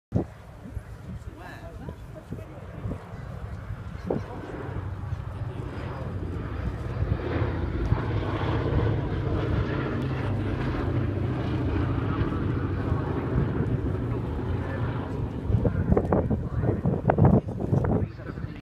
B-17 Flying Fortress flyby at Scampton Airshow
The B-17 flying over at the RAF Scampton airshow